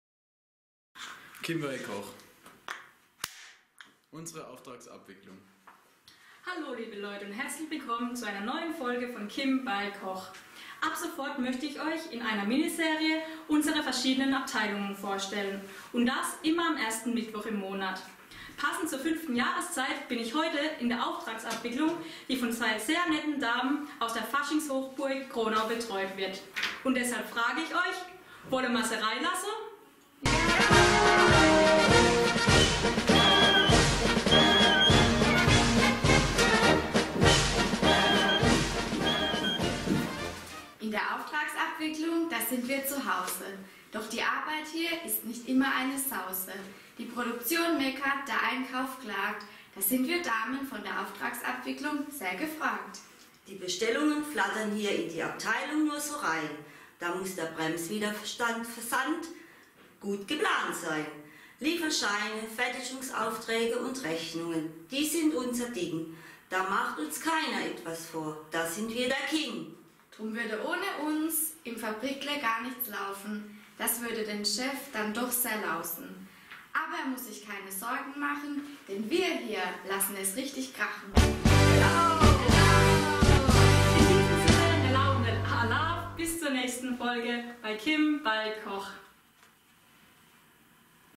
Kim bei Koch - Auftragsabwicklung
Heute in der Folge startet Kim eine Miniserie, in dieser sie jeden ersten Mittwoch im Monat eine Abteilung aus unserem Fabrikle vorstellt. Als Erstes ist die närrische Auftragsabwicklung dran. Viel Spaß beim Video und Hellau!